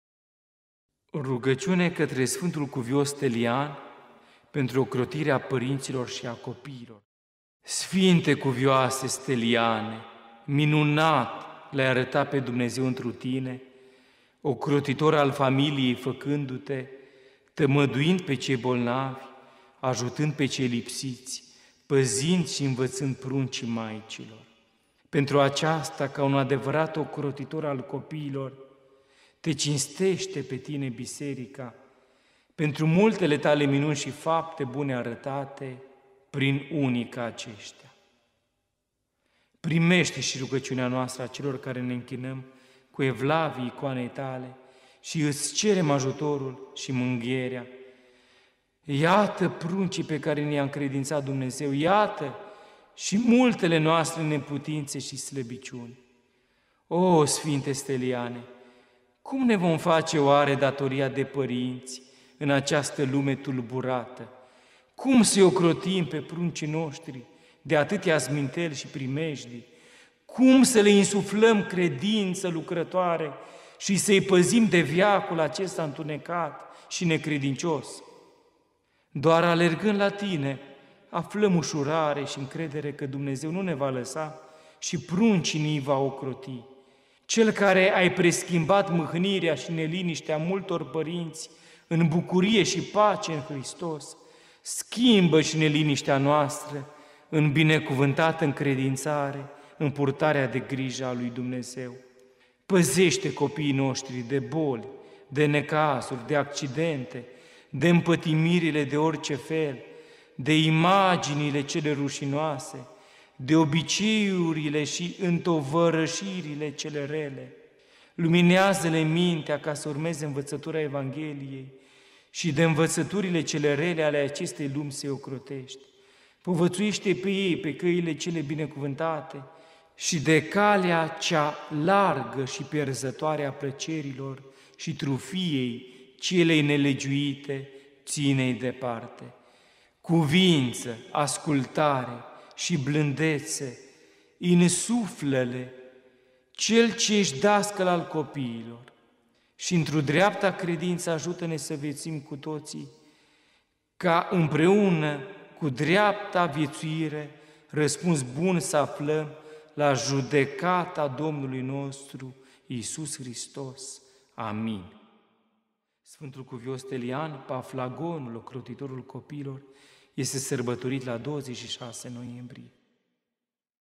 13. Rugăciune către Sfântul Cuvios Stelian, pentru ocrotirea părinților și a copiilor
Rugăciune către Sfântul Cuvios Stelian, pentru ocrotirea părinților și a copiilor
(a Episcopului Macarie Drăgoi)

Sfinte Cuvioase Steliane, minunat L-ai arătat pe Dumnezeu întru tine, ocrotitor al familiei făcându-te, tămăduind pe cei bolnavi, ajutând pe cei lipsiți, păzind și învățând pruncii maicilor. Pentru aceasta, ca un adevărat ocrotitor al copiilor te cinstește pe tine Biserica, pentru multele tale minuni și fapte bune arătate prin unii ca aceștia. Primește și rugăciunea noastră, a celor care ne închinăm cu evlavie icoanei tale și îți cerem ajutorul și mângâierea. Iată, pruncii pe care ni i-a încredințat Dumnezeu. Iată și multele noastre neputințe și slăbiciuni.
Oh, Sfinte Steliane, cum ne vom face oare datoria de părinți în această lume tulburată? Cum să-i ocrotim pe pruncii noștri de atâtea sminteli și primejdii? Cum să le insuflăm credință lucrătoare și să-i păzim de veacul acesta întunecat și necredincios? Doar alergând la tine aflăm ușurare și încredere că Dumnezeu nu ne va lăsa și pruncii ni-i va ocroti. Cel care ai preschimbat mâhnirea și neliniștea multor părinți în bucurie și pace în Hristos, schimbă și neliniștea noastră în binecuvântată încredințare în purtarea de grijă a lui Dumnezeu. Păzește copiii noștri de boli, de necazuri, de accidente, de împătimirile de orice fel, de imaginile cele rușinoase, de obiceiurile și întovărășirile cele rele. Luminează-le mintea ca să urmeze
învățătura Evangheliei și de învățăturile cele rele ale acestei lumi să-i ocrotești. Povățuiește-i pe ei pe căile cele binecuvântate și de calea cea largă și pierzătoare a plăcerilor și trufiei celei nelegiuite ține-i departe. Cuviință, ascultare și blândețe insuflă-le, cel ce ești dascăl al copiilor. Și întru dreapta credință ajută-ne să viețuim cu toții, ca, împreună cu dreapta viețuire, răspuns bun să aflăm la Judecata Domnului nostru Iisus Hristos. Amin.

Notă:
Sfântul Cuvios Stelian Paflagonul, ocrotitorul copiilor, este sărbătorit la 26 noiembrie.